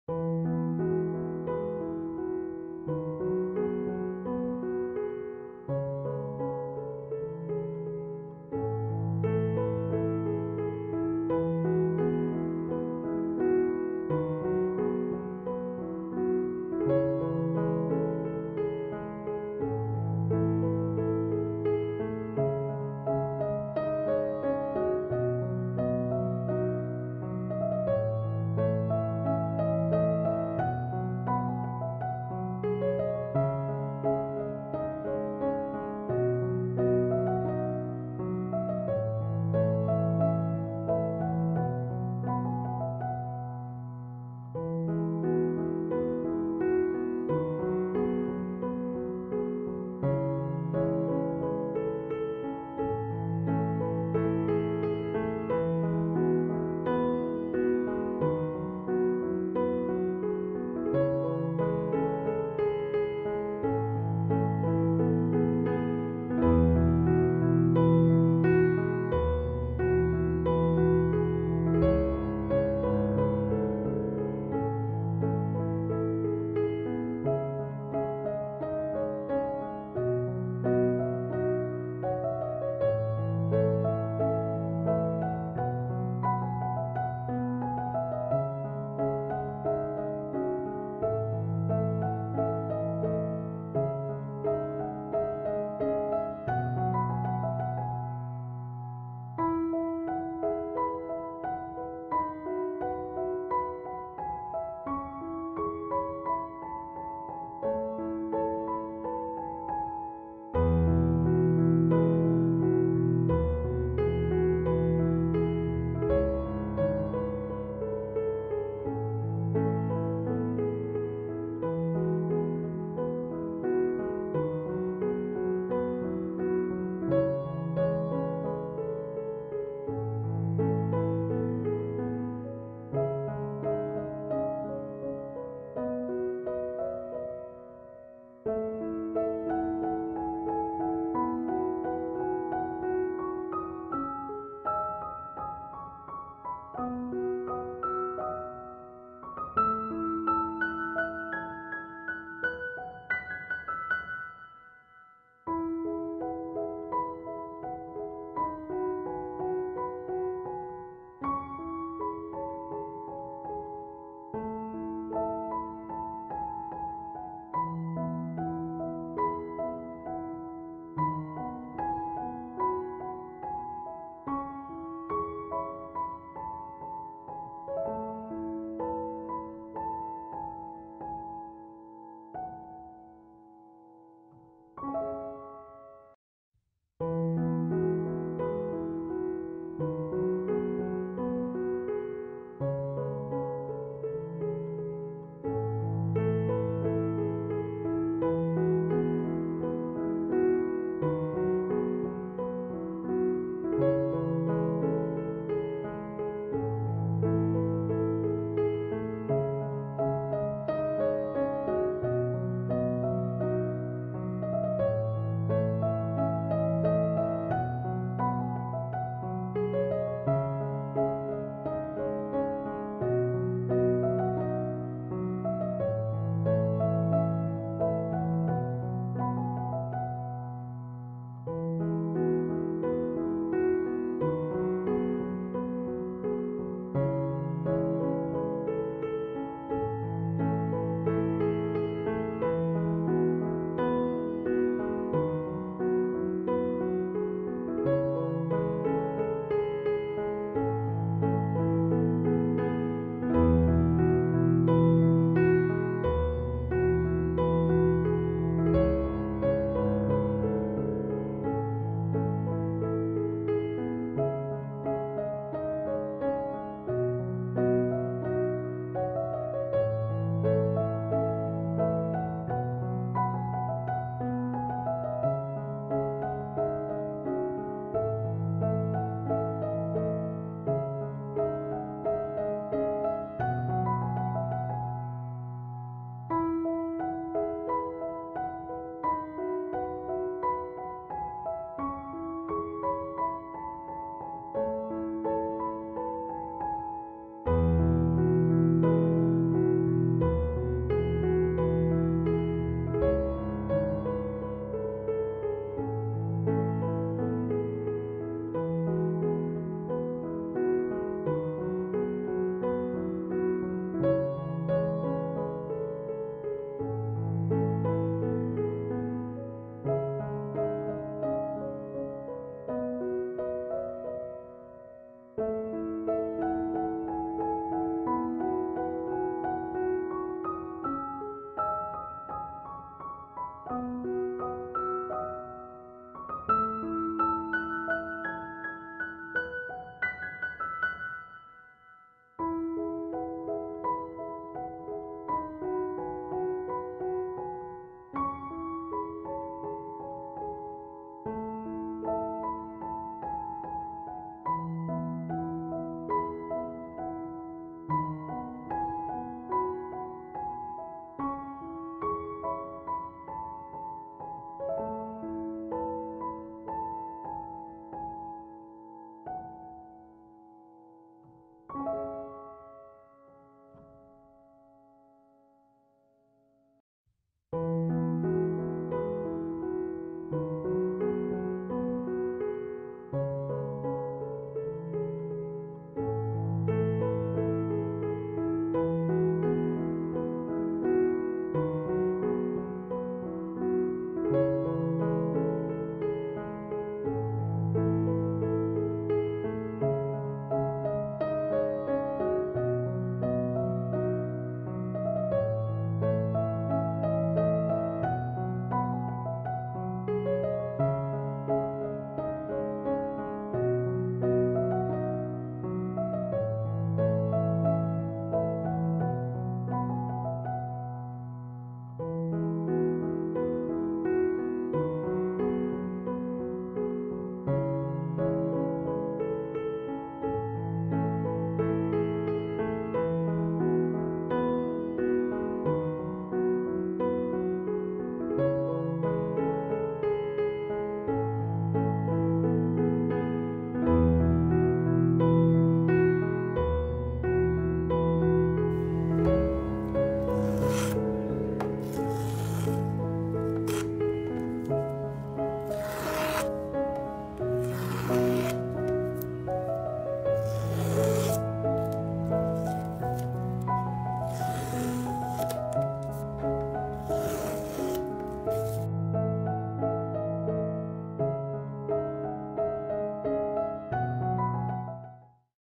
4 Seasons Tree 🌳 Acrylic Painting Tutorial for Beginners / Step By step #232 / Satisfying ASMR
Hello :-)
Today's Painting is Seasons Tree 🌳